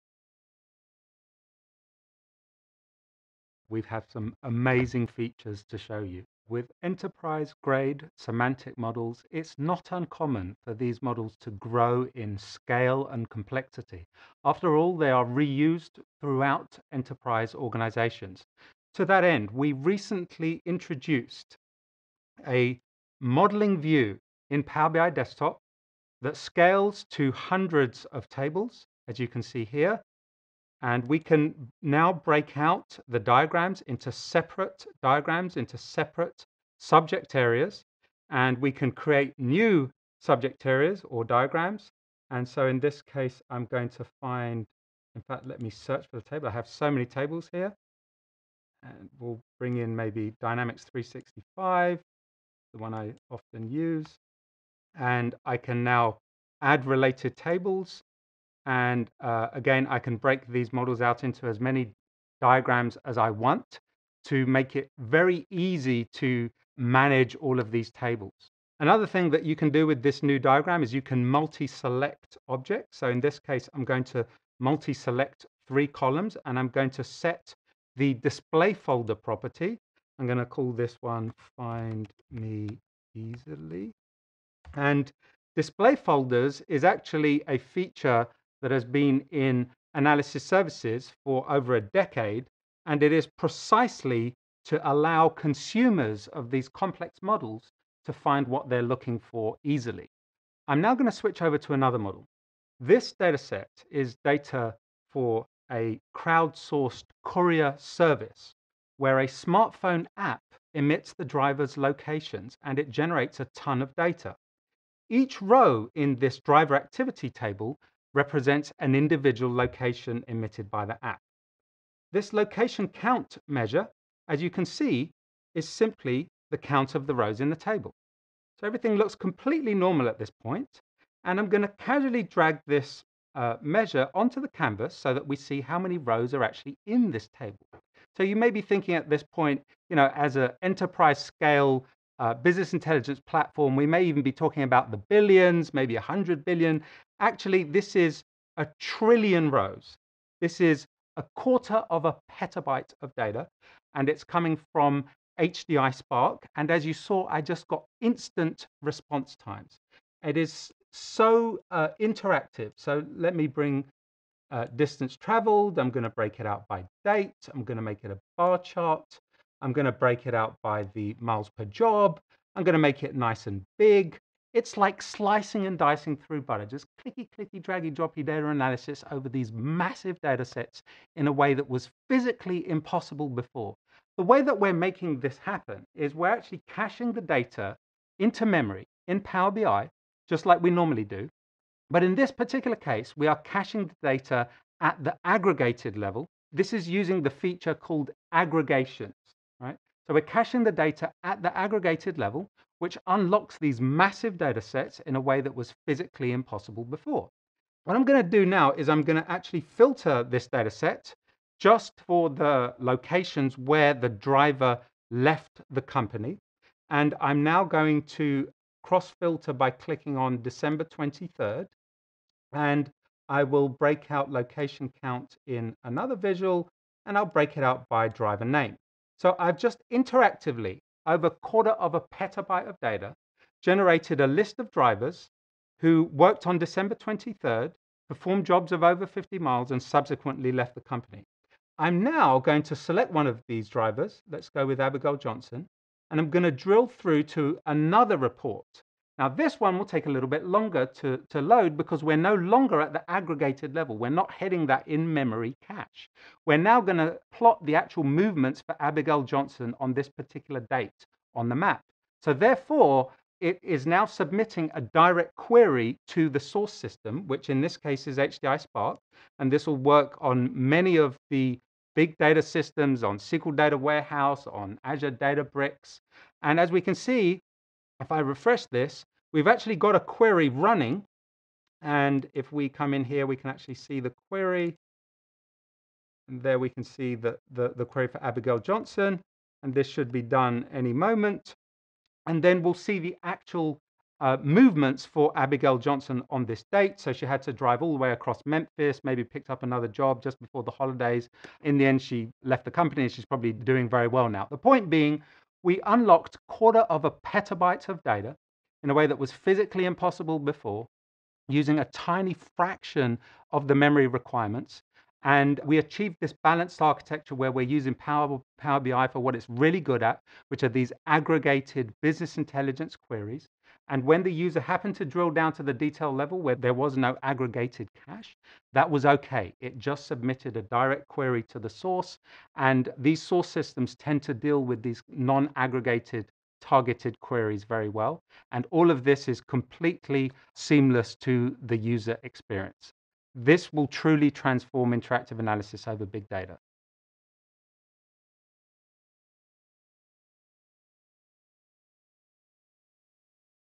PowerBI - How to understand, build, and work with semantic models
How to understand, build, and work with semantic models
Learn how the modeling view tool makes it easy to understand and build semantic models. Connect to hundreds of tables and visualize data in charts, maps, and interactive graphs at petabyte scale.  Contact ProStrategy Analytics Hub @